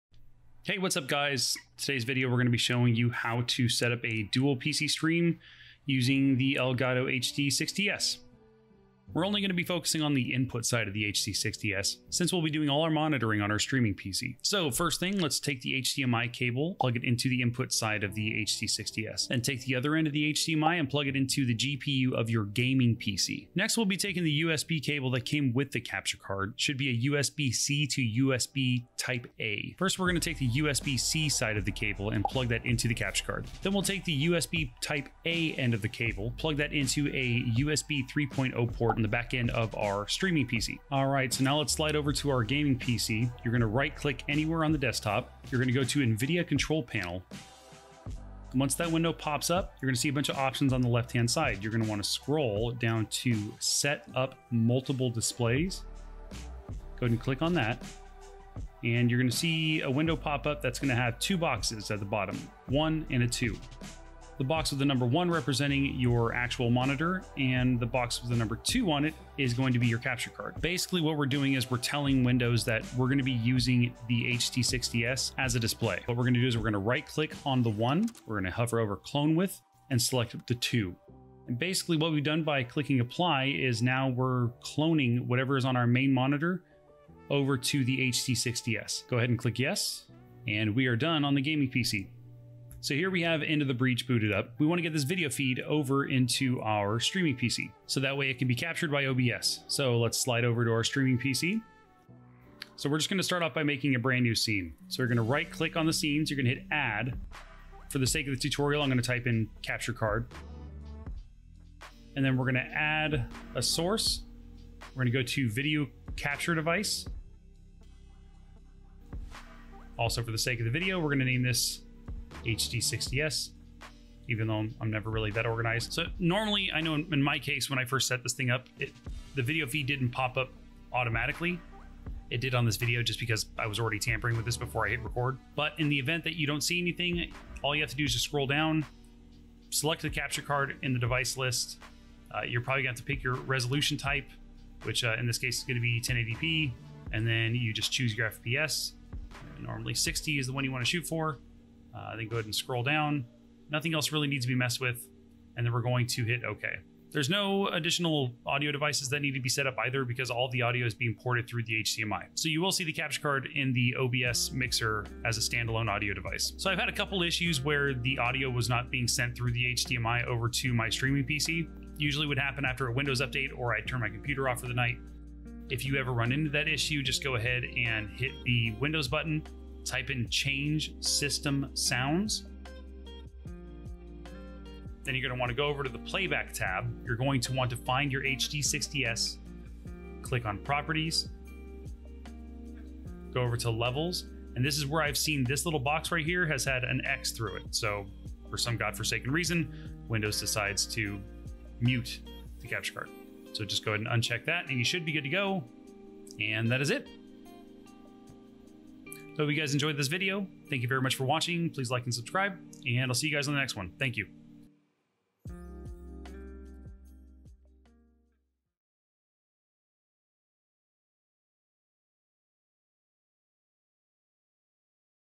How to setup Dual PC stream with the Elgato HD60s
Quick tutorial on how to setup a Dual PC stream using the Elgato HD60s. We start from how to connect the HD60s into both of your PC's, then how to set up the Nvidia Control Panel. As well as how to setup the Elgato HD60s in OBS.

If you guys prefer a written guide you can check it out over on my site: